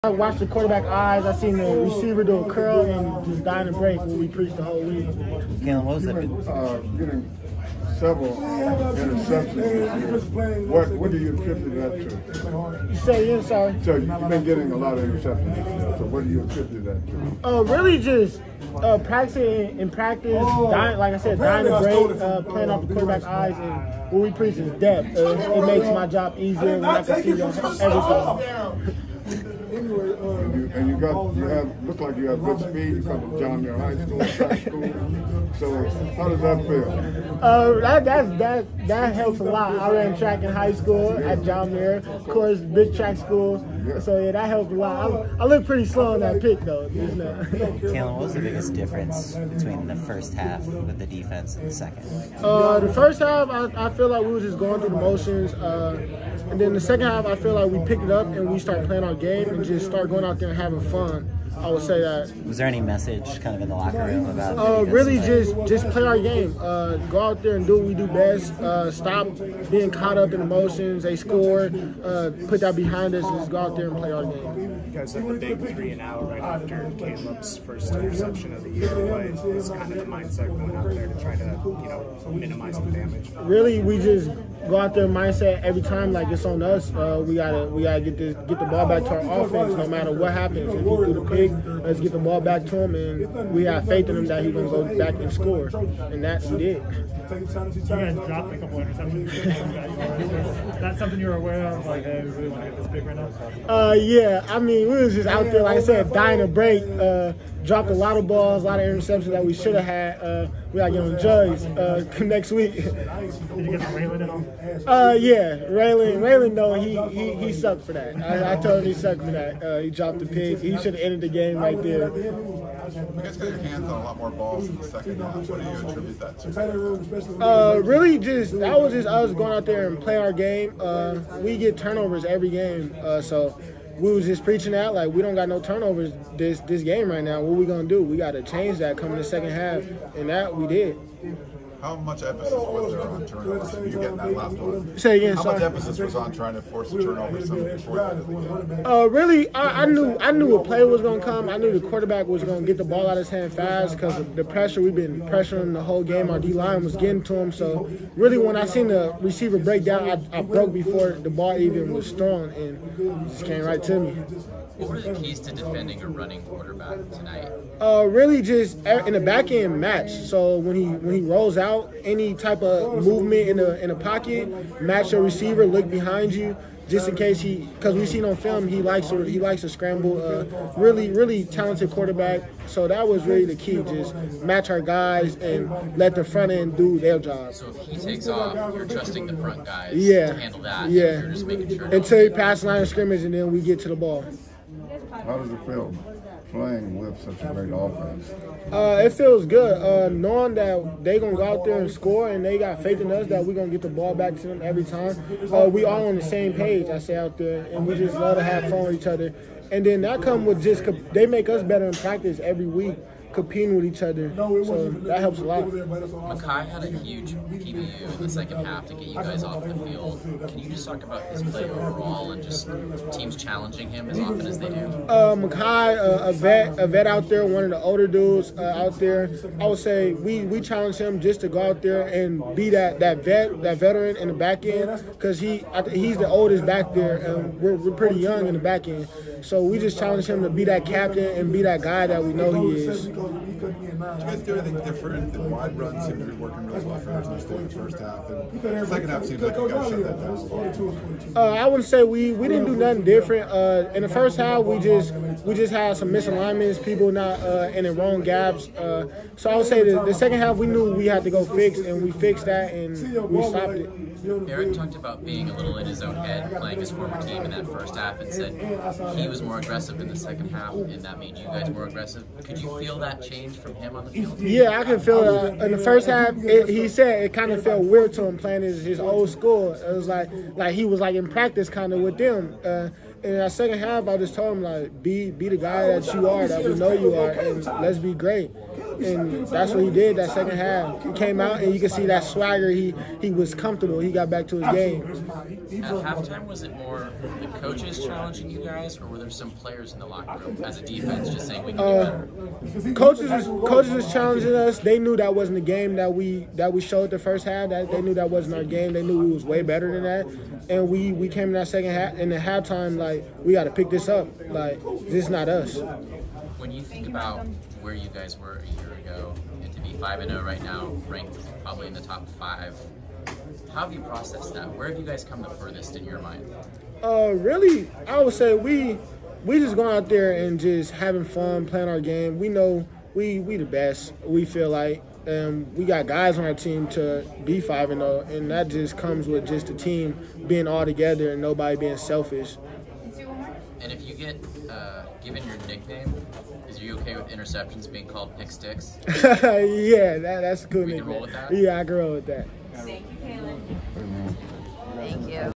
DB Calen Bullock on USC’s win over ASU, his second interception of 2022
USC DB Calen Bullock speaks to the media after USC's win against ASU

Presented by @SmartFinal1